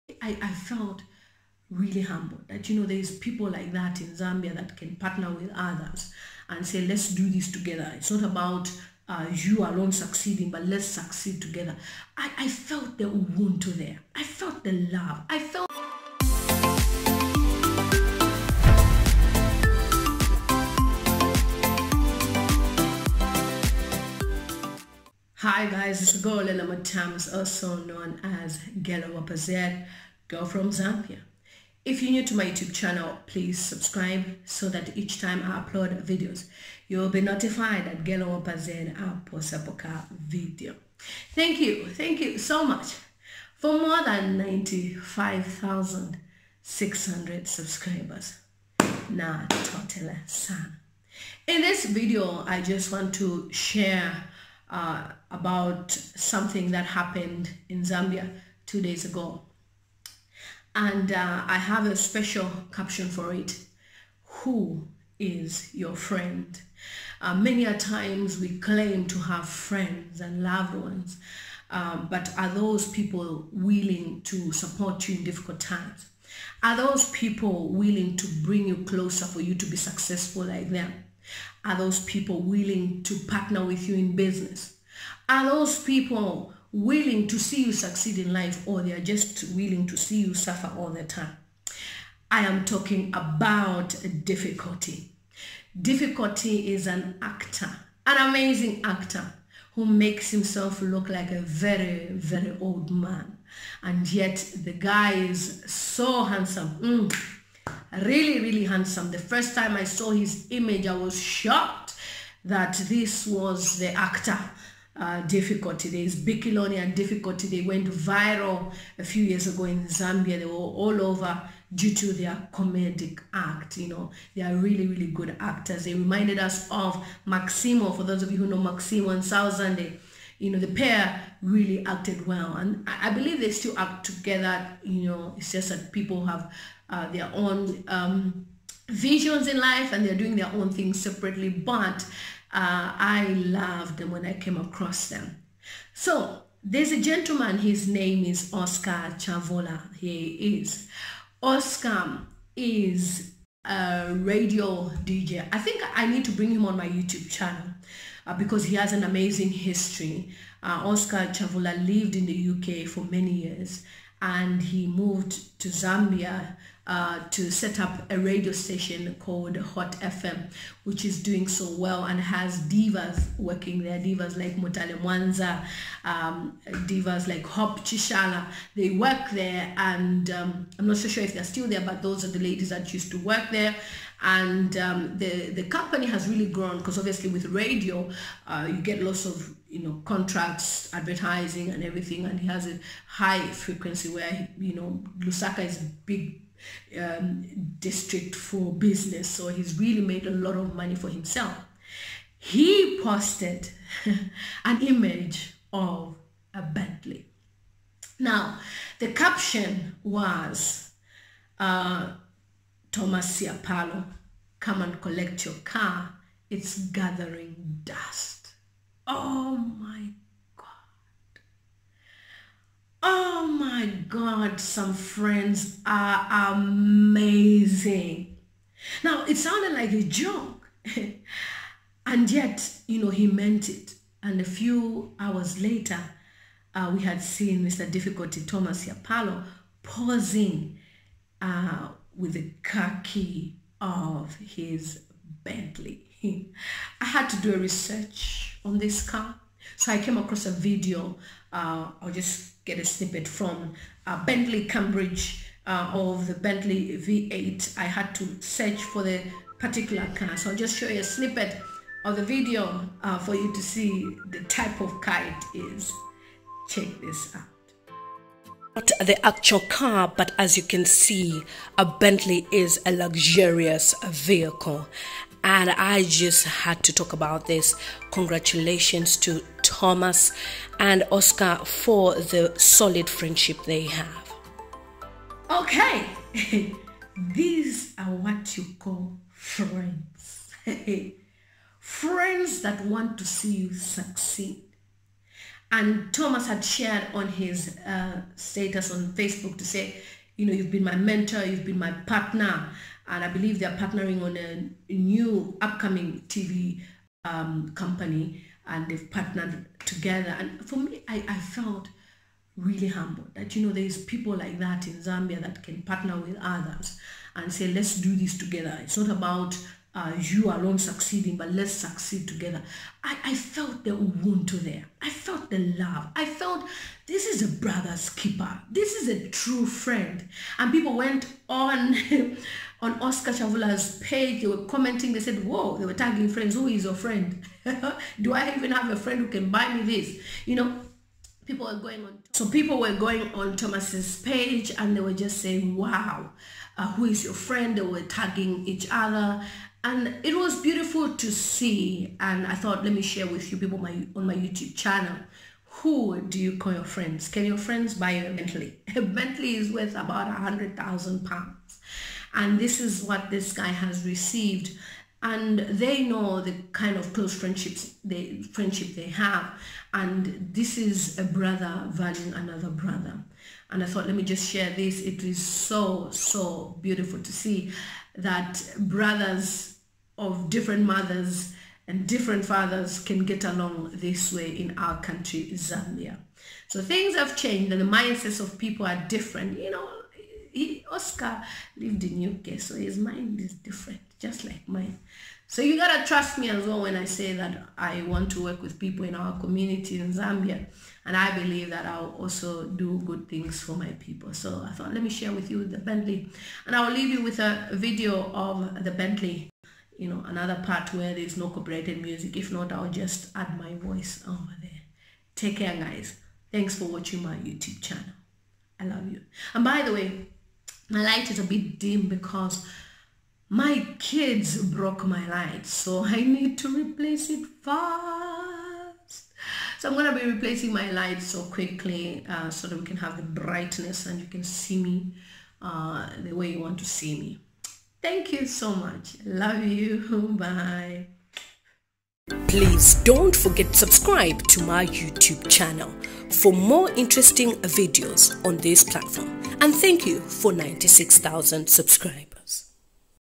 HOT FM BOSS OSCAR CHAVULA GIFTS DIFFIKOTI A BENTLEY V8
This one is a powerful gift from a friend. Amazing.

To support me please use